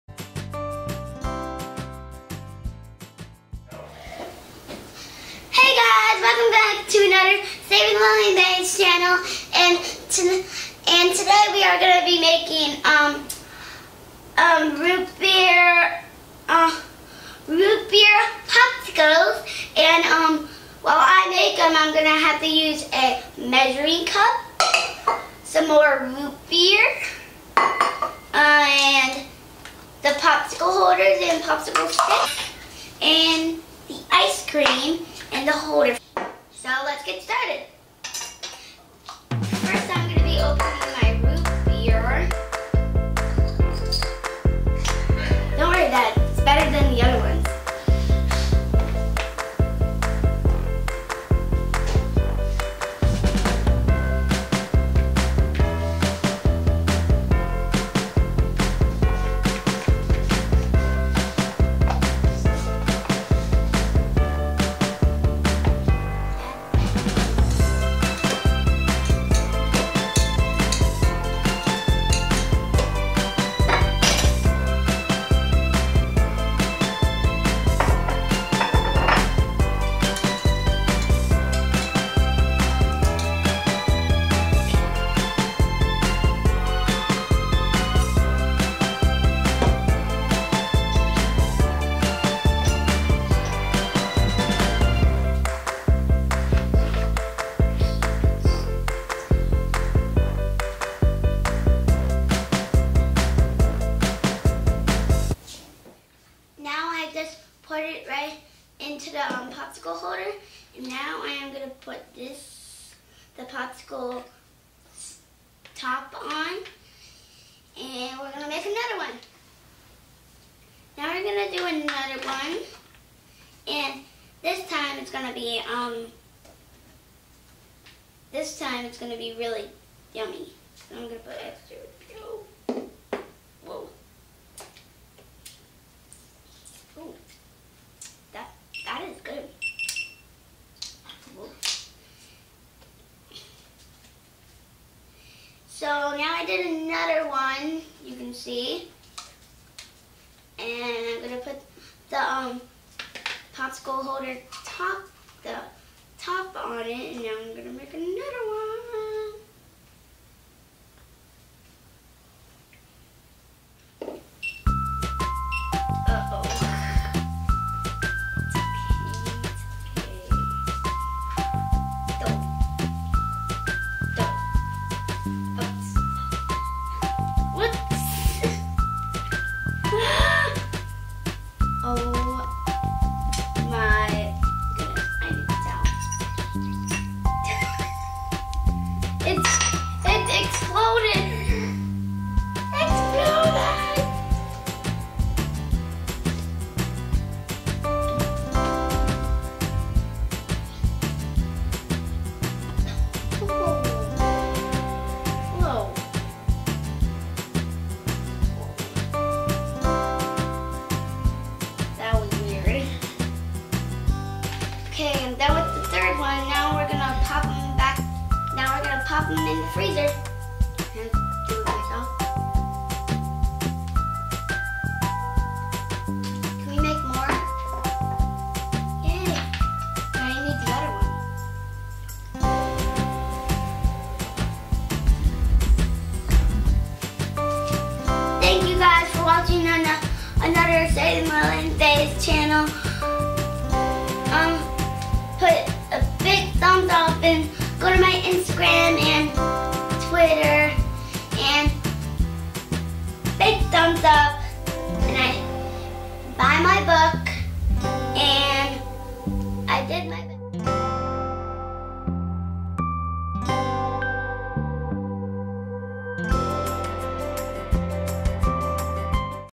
How Lilly Makes her Root Beer Float Popsicles with Ice Cream Easter
Lilly brings her funny style to Easter Edition how she makes her ROOT BEER Float Popsicles a long time favorite for the up coming summer.
She uses The 1924 blend Stewarts original fountain classic Root Beer  make sure it is cold draft and in the cooler before you pour it!!! Second is the Ice Cream Blue Bell Vanilla Homemade Lilly says a fan fav!

Also follow Lilly on BEME @ SavingLillianBay  (Working on app)